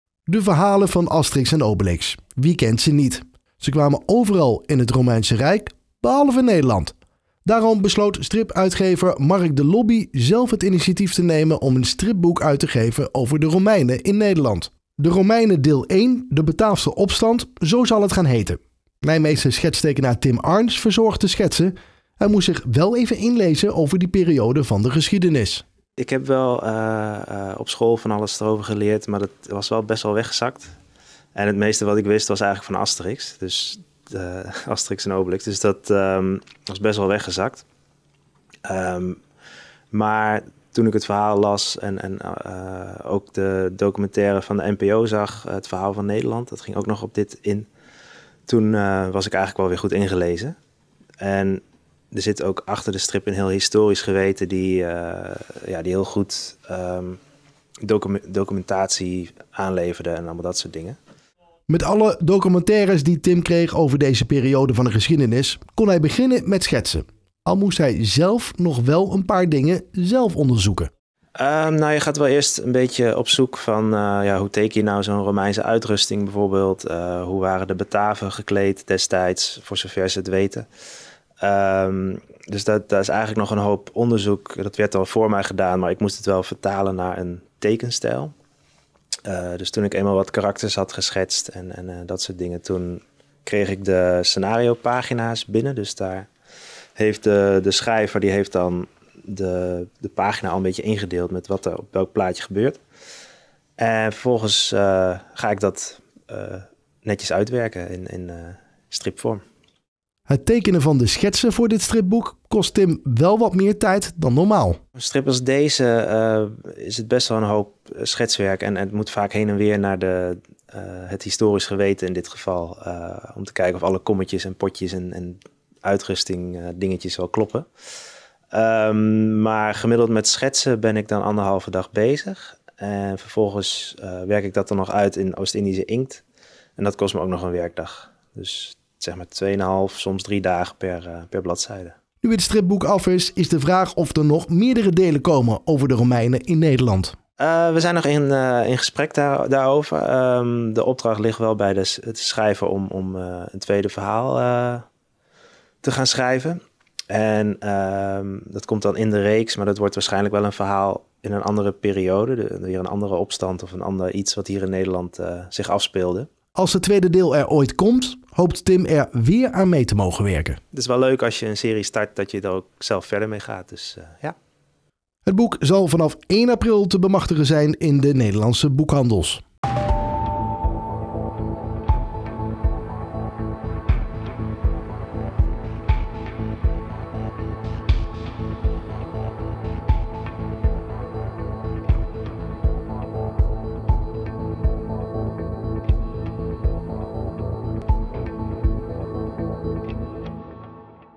Asterix & Obelix bezoeken nu óók Romeins Nederland || RN7 REGIONIEUWS
De verhalen van Asterix en Obelix, wie kent ze niet. Ze kwamen overal in het Romeinse Rijk behalve in Nederland. Daarom besloot stripuitgever Marc de Lobie zelf het initiatief te nemen om een stripboek uit te geven over de Romeinen in Nederland. De Romeinen deel 1- De bataafse opstand zal het gaan heten.